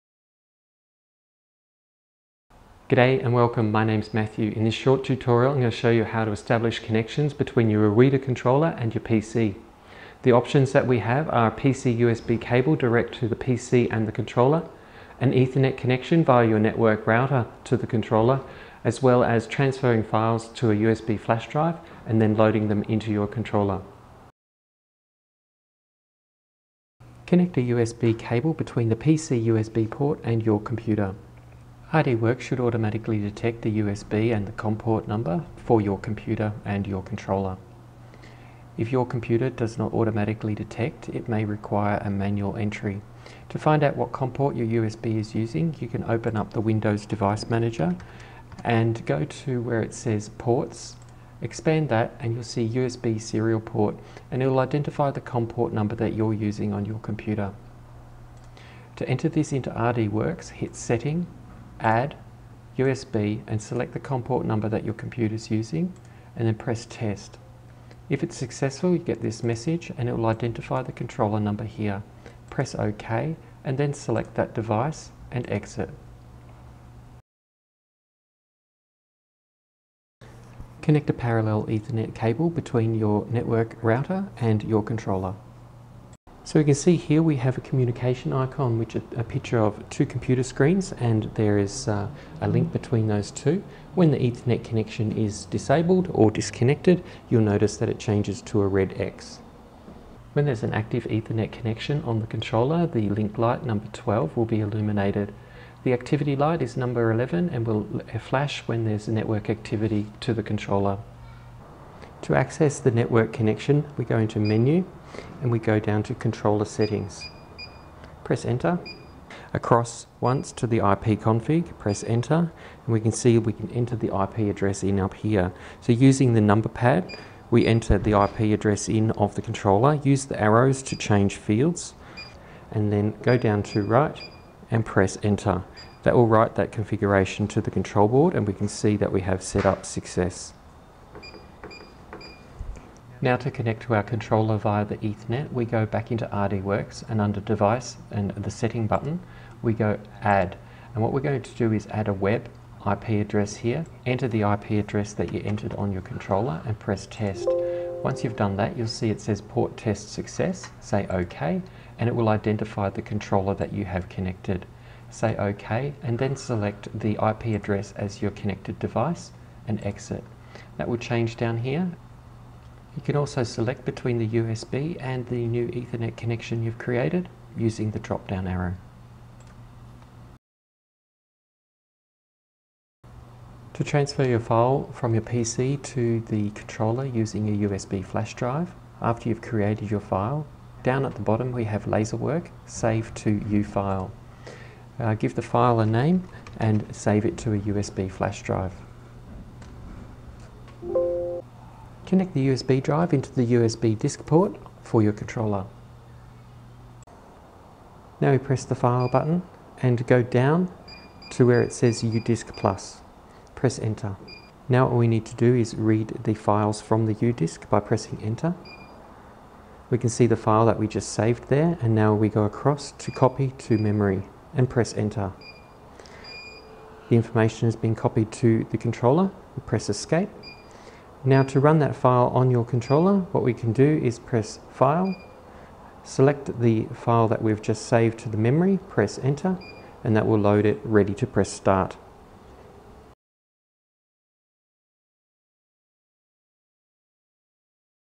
[02] RuiDa Controller - Establishing Connections - PC-USB - Ethernet - USB Disk
In this short tutorial I demonstrate how to establish connections between RD Works and Your RDC Controller.  For this demonstration I used the RDC6445.

NOTE: This is not a paid production or advertisement.  The video was produced for Ruida & Cloudray Laser to provide new laser users with an installation / how-to video.

Ruida Controllers available at Cloudray Laser